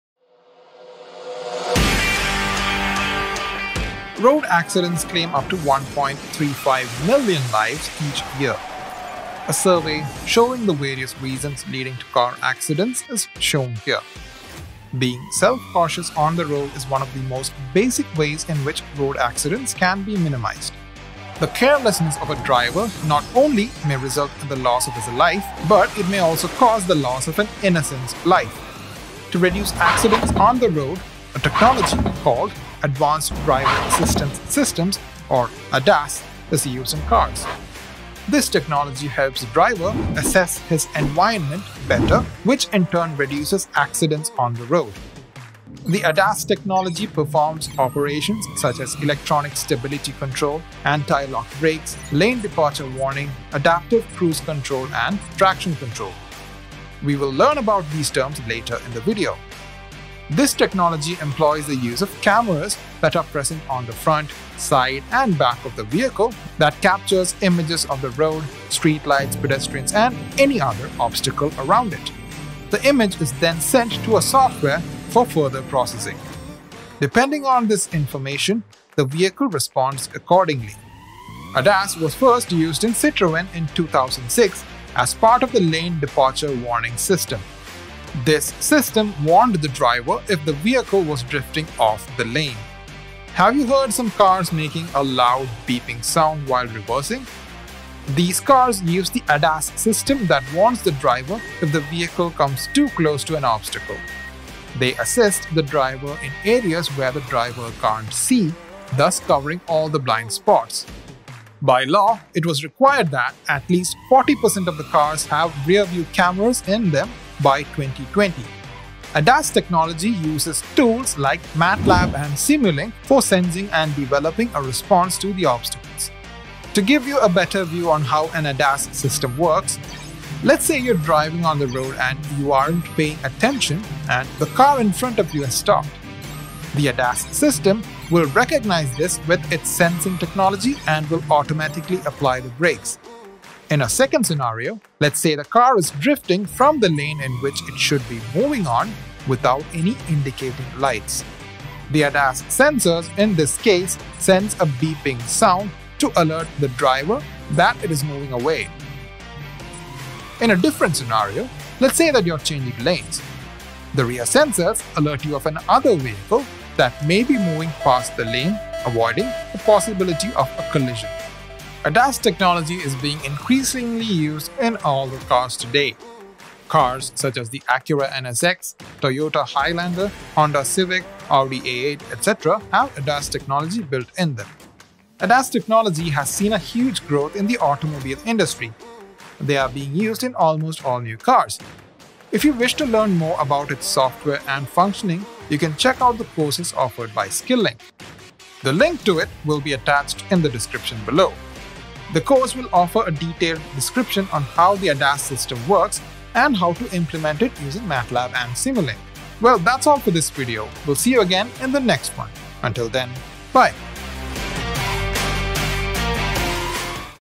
Get Started with ADAS using MATLAB & Simulink | Skill-Lync
Very often, road accidents are caused due to the driver not paying attention on the street. Incidents such as these, are a threat to the life of both driver and the person present on the spot of the accident.

To reduce this and increase the convenience of the driver, the Advanced Driver Assistance System is used.

ADAS with the help of sensors can detect if there is an obstacle in front of it and stop, it also alerts the driver if the car has drifted off the lane, other than this, it also informs the driver if another car is crossing it.

Today, almost 40 per cent of cars have ADAS systems in them.

If you are someone who wants to learn this system in detail and how it works, click the

References:

IFCAR / Public domain
Bull-Doser / Public domain